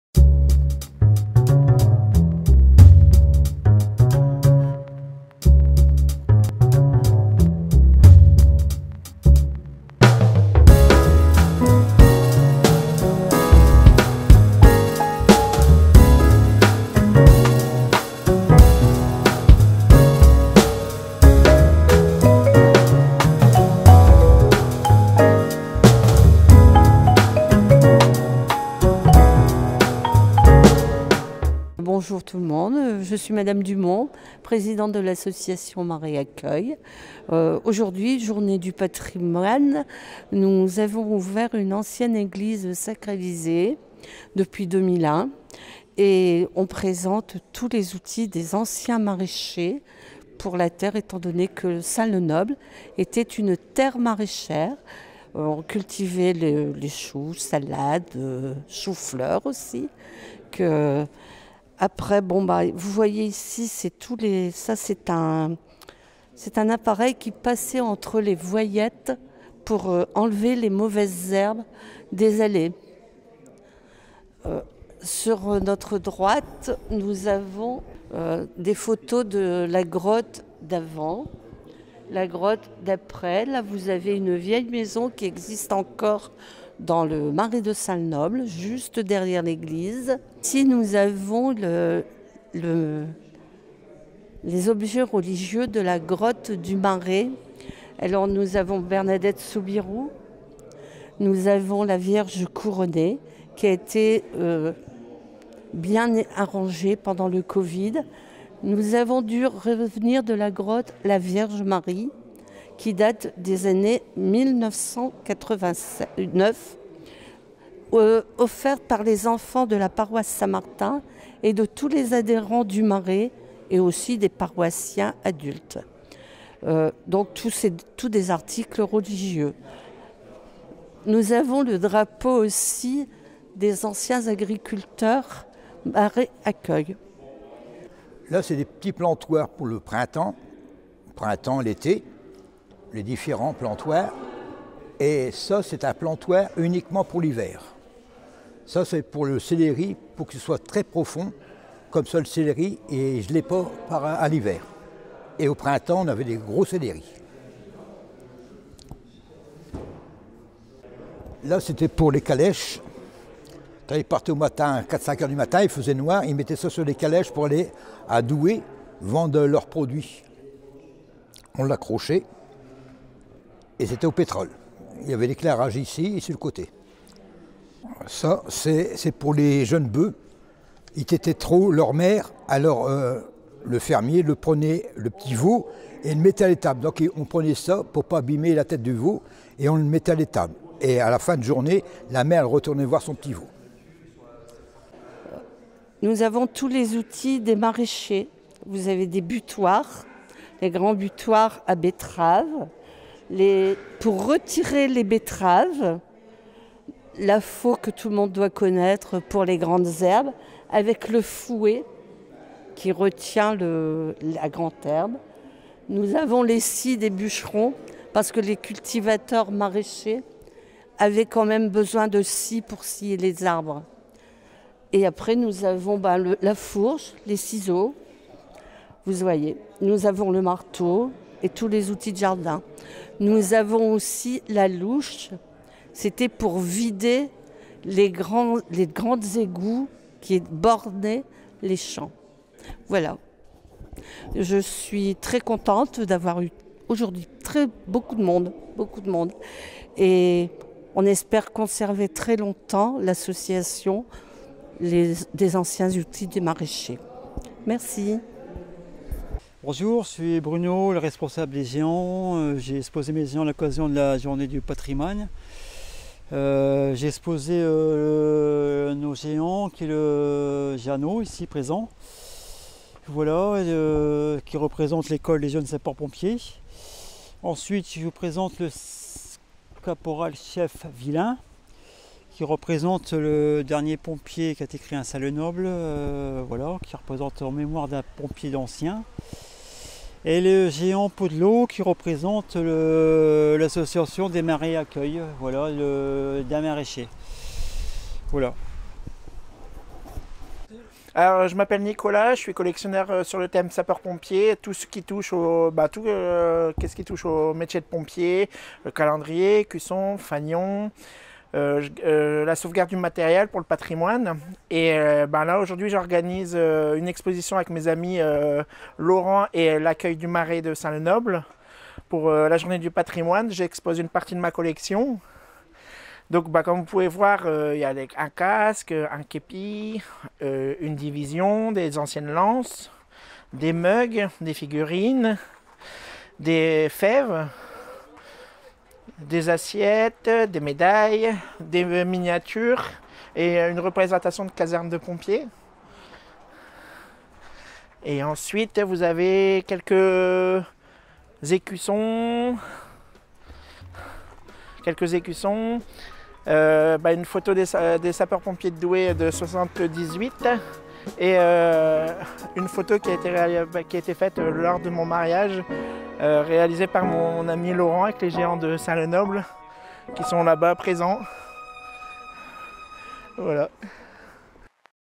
Marais Accueil - Journée du Patrimoine Sinois 2022
Expositions proposées par l'association "Marais Accueil" et les pompiers autour d'outils et des machines anciennes de maraichage, de photos du quartier du Marais et également une belle exposition d'accessoires et casques de pompiers.